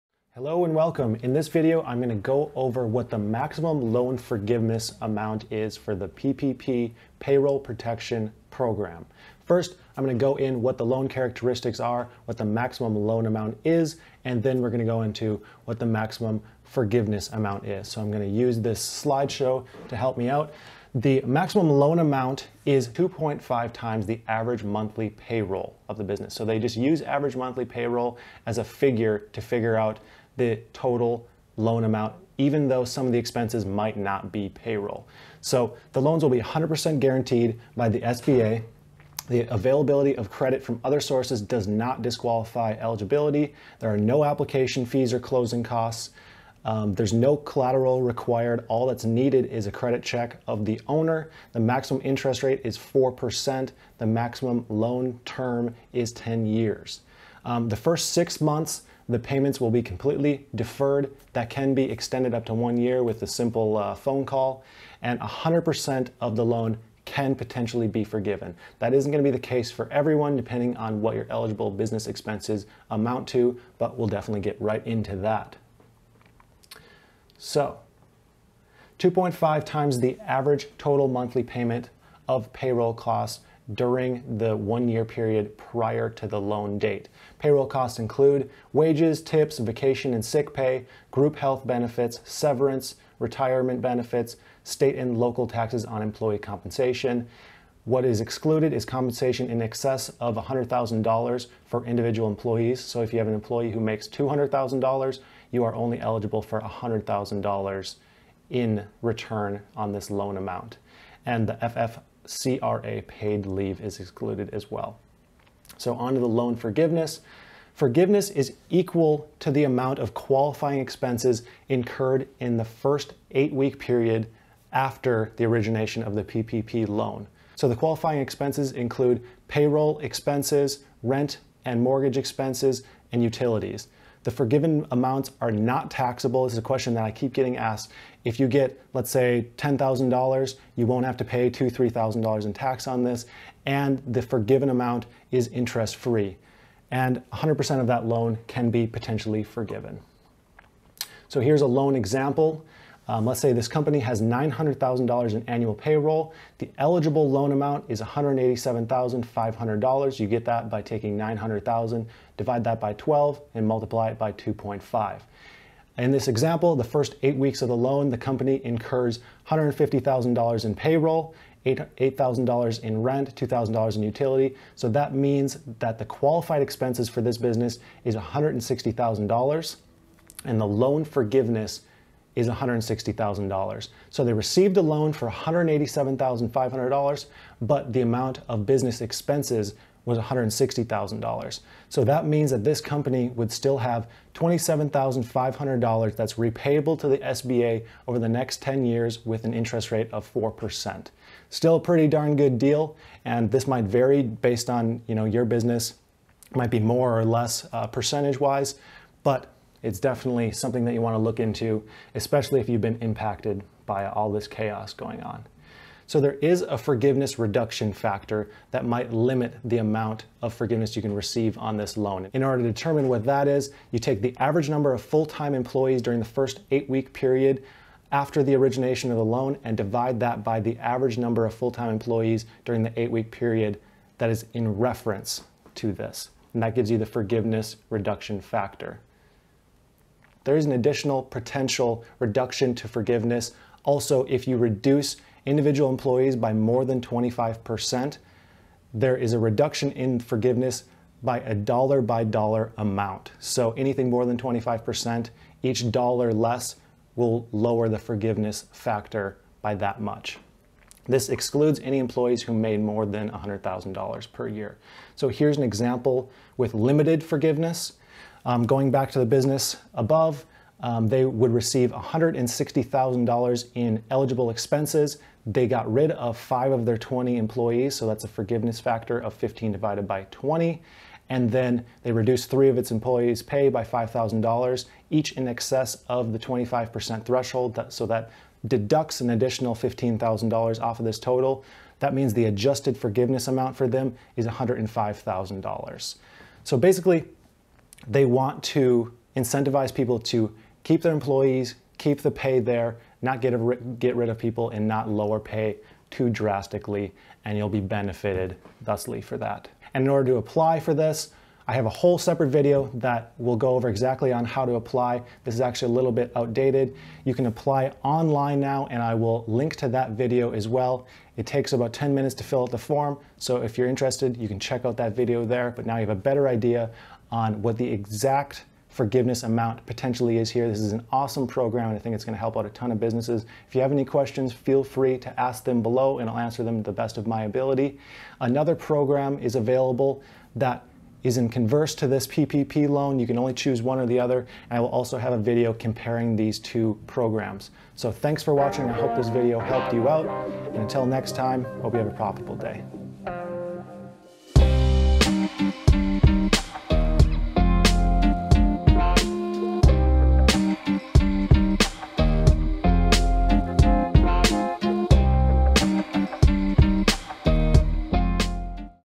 Maximum Loan Forgiveness Amount For Payroll Protection Program PPP
See how large of a forgivable loan you are eligible for! Download the Paycheck Protection Program PPP and Employee Retention Tax Credit Calculator

list of PPP lenders that don't require an existing bank account- download

Get emailed to be notified of new posts and exclusive content. No sales. No BS.

What is PPP Paycheck Protection Program?
- Establishes $350 Billion for the provision of loans to small business
- New type of SBA Section 7(a) loans
- For loans made between February 20, 2020 and June 30, 2020
- Only available to qualifying "small" employers
- Up to 100% of the loan amount is eligible for forgiveness

Paycheck Protection Loan Criteria
- Maximum loan amount is lesser of (a) 2.5 times the average monthly payroll (based on 12-month lookback) or (b) $10 million
- Loans will be 100% guaranteed by the SBA
- Availability of credit from other sources does not disqualify eligibility
- No application fees or closing costs
- Nonrecourse; no collateral or personal guarantees imposed
- Maximum interest rate is 4%
- Maximum loan term of 10 years
- First 6 months of payments (principal and interest) are automatically deferred;
deferral can extend up to a year
- Up to 100% of the PPP loan can be forgiven

Maximum Loan From the Paycheck Protection Program
- 2.5 times average total monthly payments of “payroll costs” during the 1-year period
before date the loan is made.

How Does Loan Forgiveness Work
- Forgiveness equal to amount of qualifying expenses incurred in the first 8-week
period after origination of the PPP loan.
- “Qualifying expenses” include:
      Payroll costs (defined earlier)
      Rent payments
      Mortgage interest (no principal payments)
      Utility payments (electricity, gas, water, transportation, telephone, or internet
     access)
- Forgiven amounts are not taxable to the small business (i.e., a tax-free grant)
- Forgiven amounts are interest free

Paycheck Protection Limits on Loan Forgiveness
- Loan forgiveness is reduced proportionately to the reduction in employee headcount during the COVID-19 crisis.
- Loan forgiveness is reduced dollar-for-dollar by decreases in an employee’s compensation greater than 25% from prior quarters

How To Apply For a PPP Paycheck Protection Loan

Mic I Use
4k Camera I Use